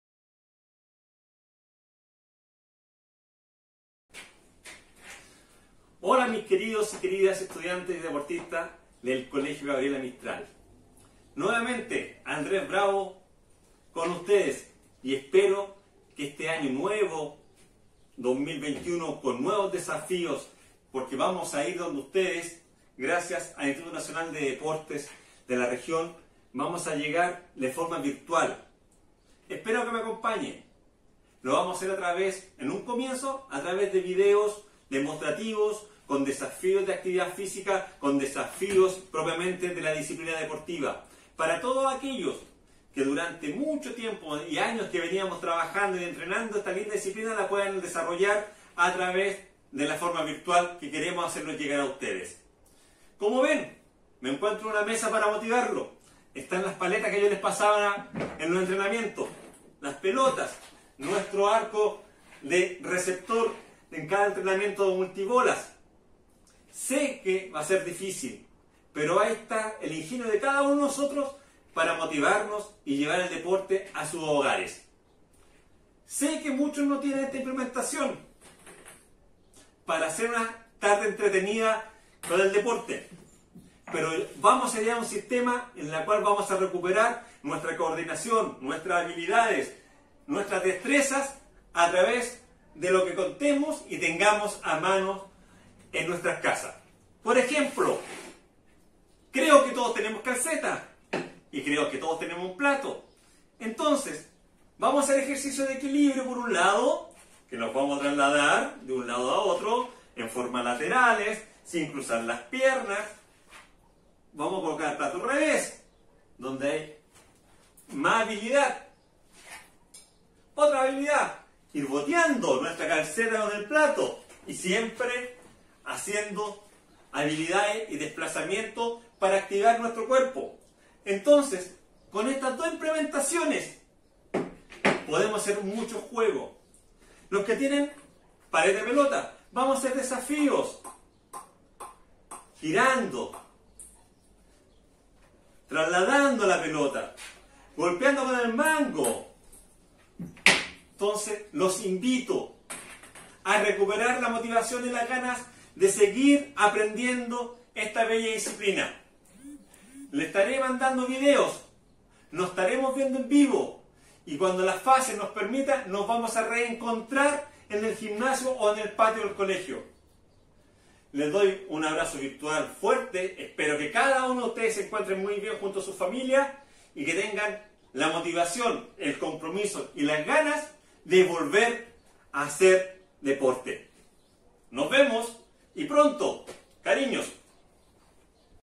Video N 1: invitación a taller
Te invitamos a ser parte del primer taller on line de tenis de mesa, una alianza entre IND y colegio Gabriela Mistral, a través de su programa Crecer en Movimiento.